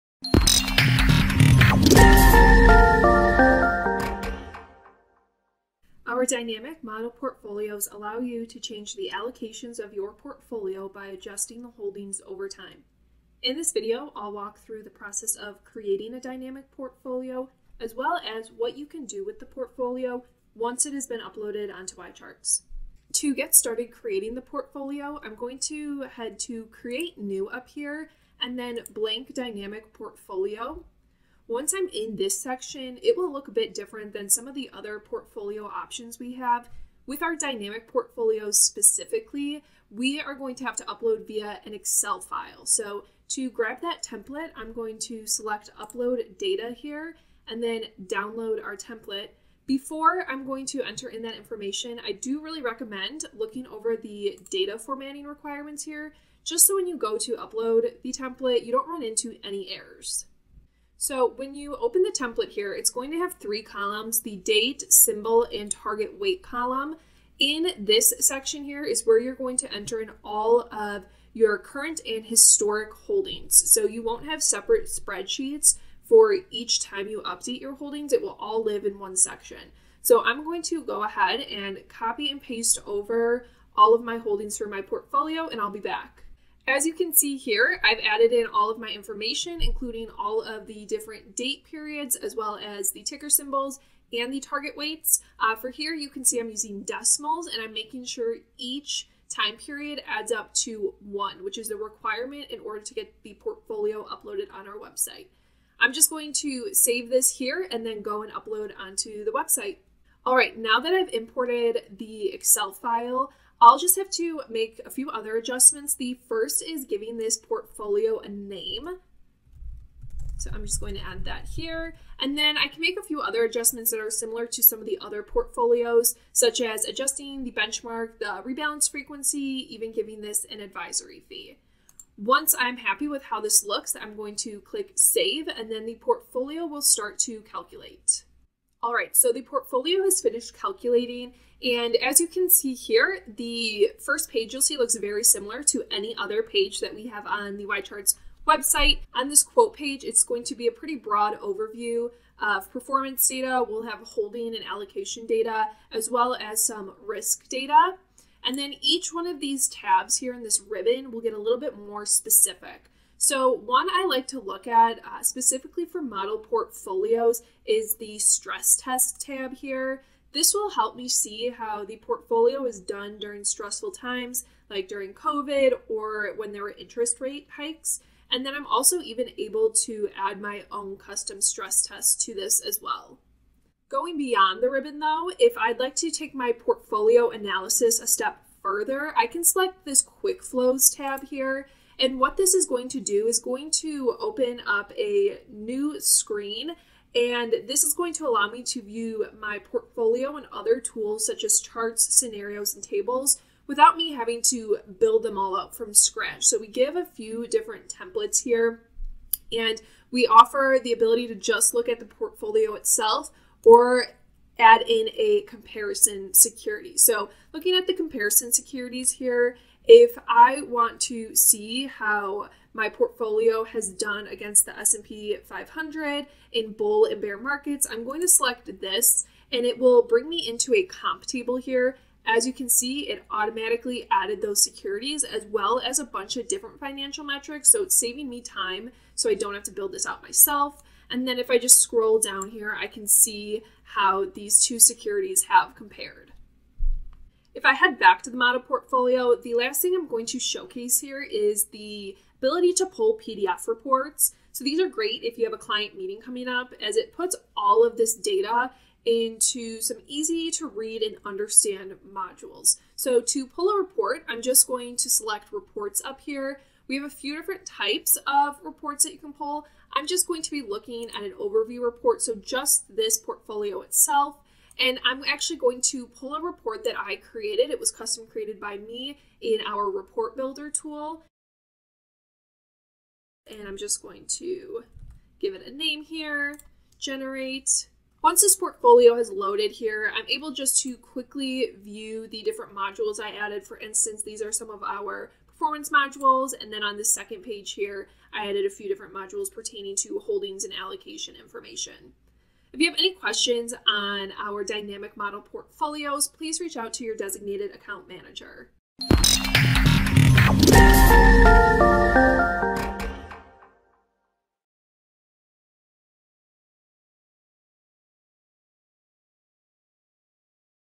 YCharts 101: Getting Started with Dynamic Model Portfolios
Explore Dynamic Model Portfolios, a tool that can upload dynamic holdings data to build model strategies, import client holdings, analyze portfolio data, and generate PDF reports.

This 5-minute demo walks through how to create a Dynamic Model Portfolio and leverage it throughout YCharts.

Introduction - 0:00
Creating a new Blank Dynamic Portfolio- 0:23
Downloading the Excel Template - 0:38
Going over the Upload Template - 1:01
Importing and Saving the Portfolio - 1:50
Quote Page Tab - 2:29
Stress Test Tab - 2:50
Quickflows - 3:03
Generating Portfolio Reports - 4:12
Contact Us -  5:02